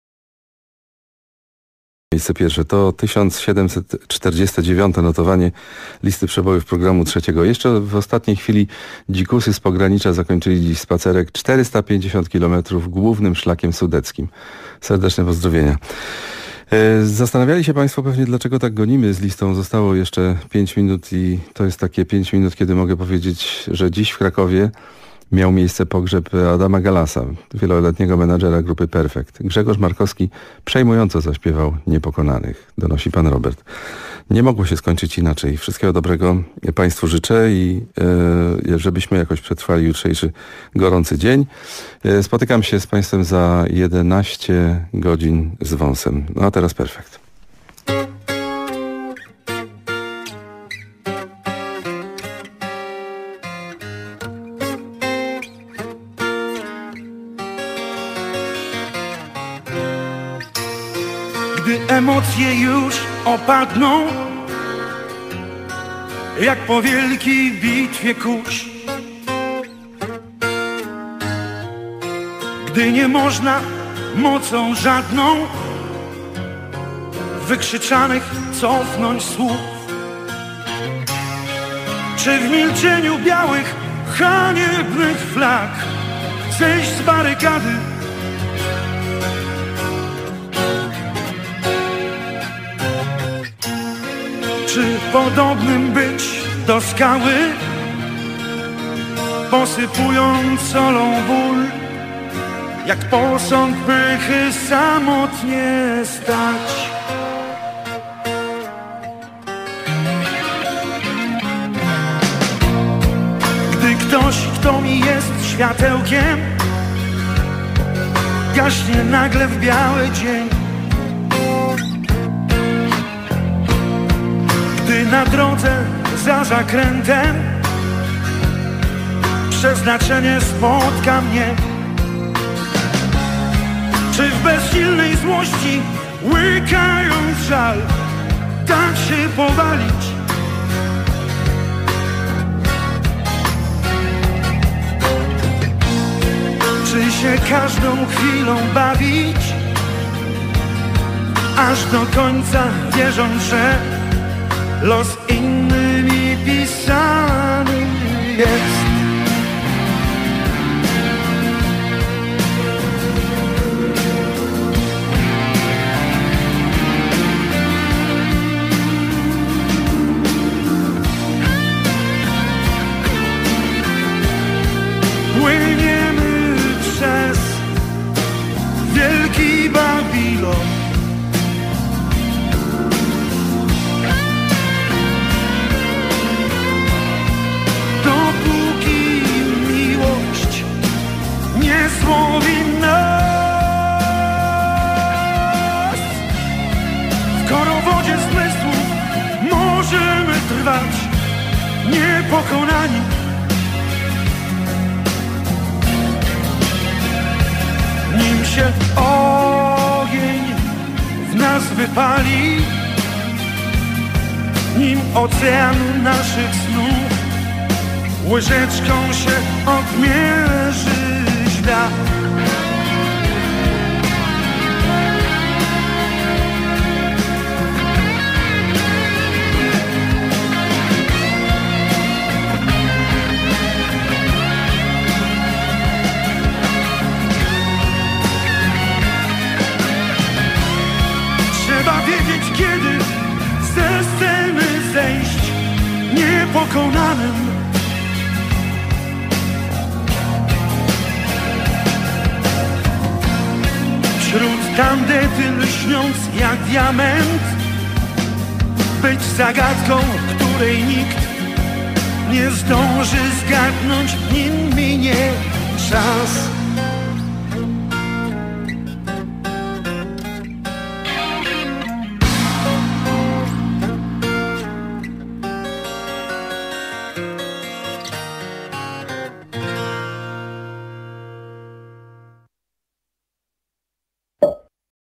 Adam Galas 1952 - 2015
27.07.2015 zmarł manager zespołu Perfect Adam Galas (63 l.).